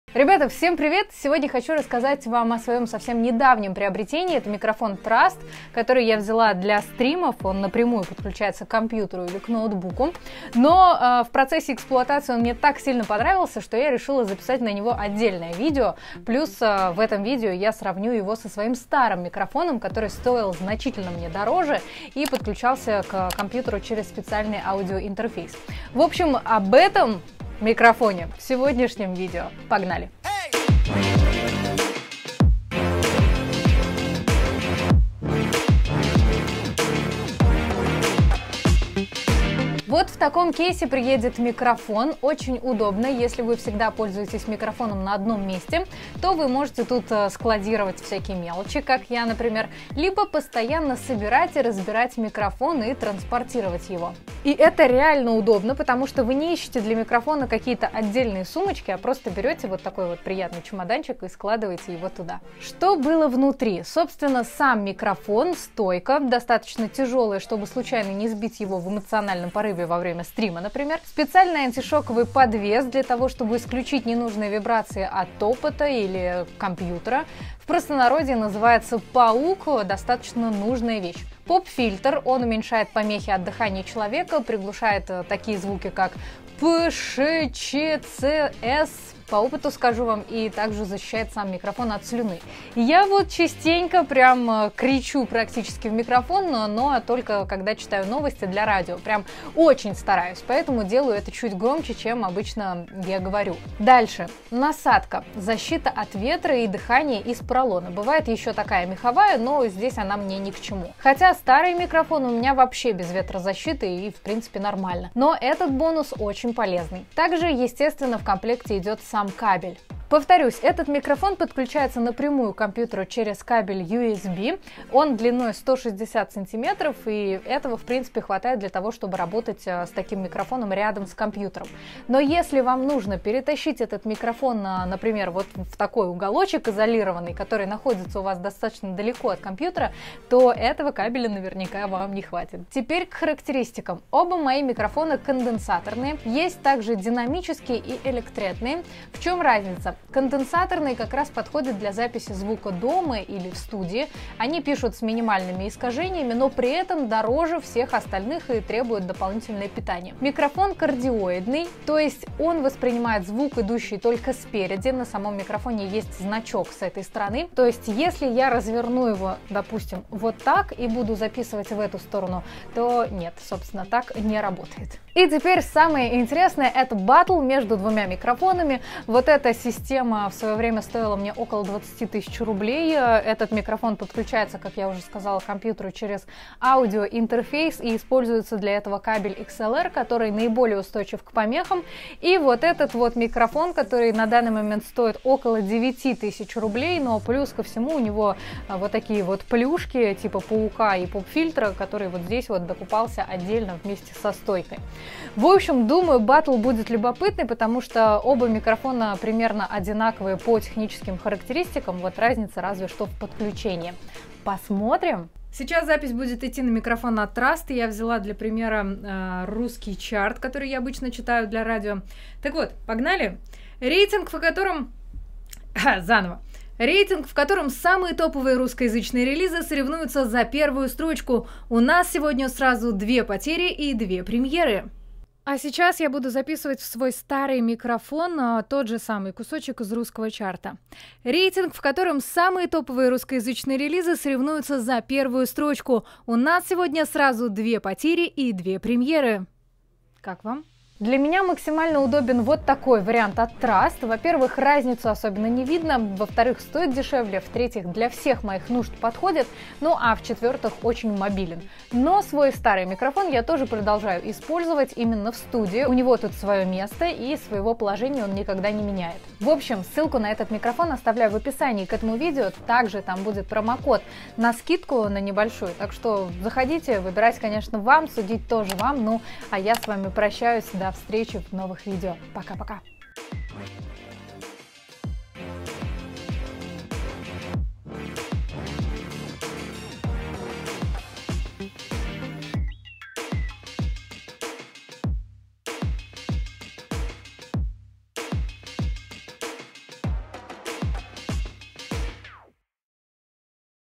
Лучший микрофон для записи голоса, стрима от Trust GXT 252 Emita
Ребята, не ожидала, но этот микрофон стоит того, чтобы быть с вами, если вы как раз ищите себе что-то такое.
Его цена 8990 рублей на момент записи видео и это вместе с кексом, поп-фильтром, стойкой и паучком.
Микрофон идеально подходит для записи голоса у компьютера, проведения стримов дома, а не только на студии.
Без прекрас, лучшее предложение на рынке в 2019 году.